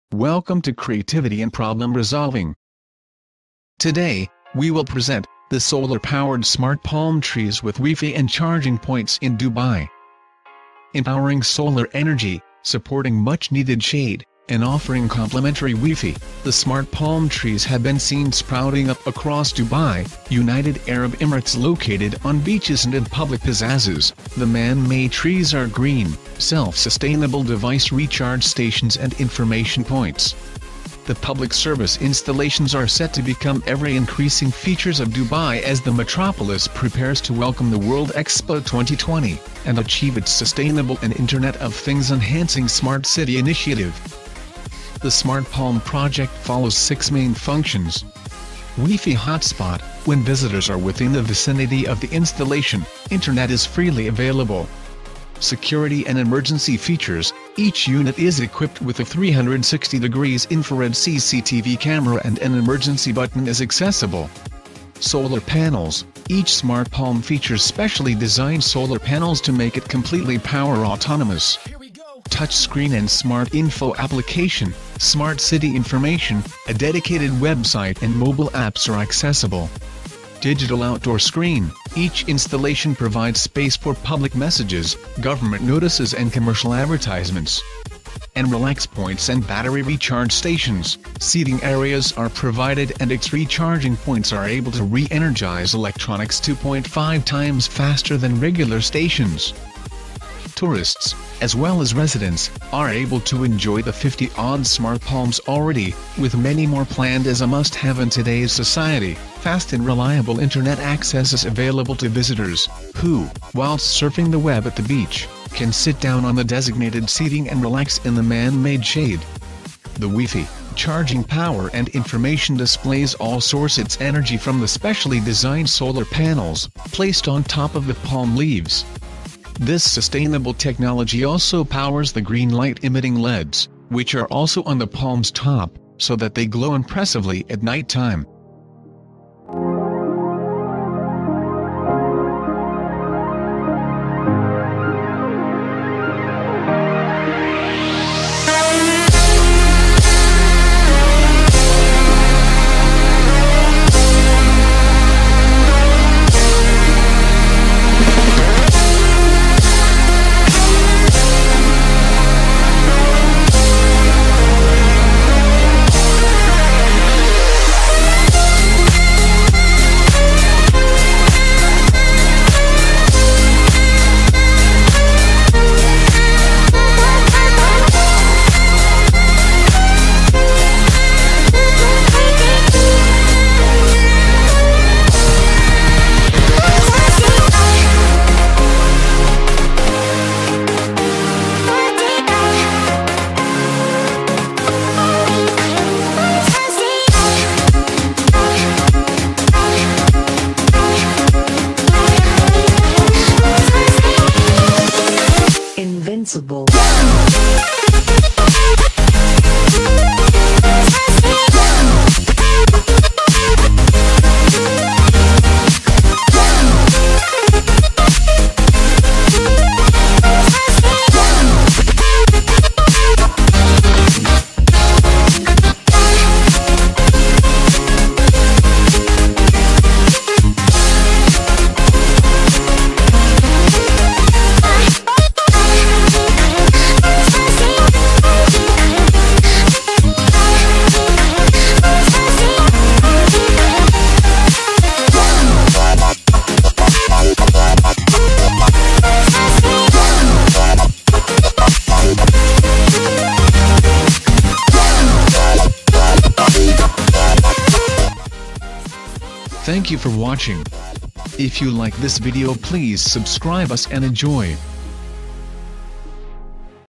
Solar powered smart palm trees with wifi & charging points in dubai
Empowering solar energy, supporting much-needed shade, and offering complimentary wifi , the ‘smart palm’ trees have been seen sprouting up across dubai, united arab emirates. located on beaches and in public pizazzes, the man-made trees are green, self-sustainable device recharge stations and information points. the public service installations are set to become every-increasing features of dubai as the metropolis prepares to welcome the world expo 2020, and achieve its sustainable and internet of things-enhancing ‘smart city’ initiative.

the ‘smart palm’ project follows six main functions; wifi hotspot – when visitors are within the vicinity of the installation, internet is freely available; security and emergency features – each unit is equipped with a 360 degrees infrared CCTV camera and an emergency button is accessible; solar panels – each ‘smart palm’ features specially designed solar panels to make it completely power autonomous; touch screen and smart info application – smart city information, a dedicated website and mobile apps are accessible; digital outdoor screen – each installation provides space for public messages, government notices and commercial advertisements; and relax points and battery recharge stations – seating areas are provided and its recharging points are able to re-energize electronics 2.5 times faster than regular stations.

tourists, as well as residents, are able to enjoy the 50-odd ‘smart palm’s already, with many more planned. as a must-have in today’s society, fast and reliable internet access is available to visitors, who, whilst surfing the web at the beach, can sit down on the designated seating and relax in the man-made shade. the wifi, charging power and information displays all source its energy from the specially designed solar panels, placed on top of the palm leaves. this sustainable technology also powers the green light-emitting leds, which are also on the palm’s top, so that they glow impressively at night time.

Smart Palm Trees Powering Beaches in Dubai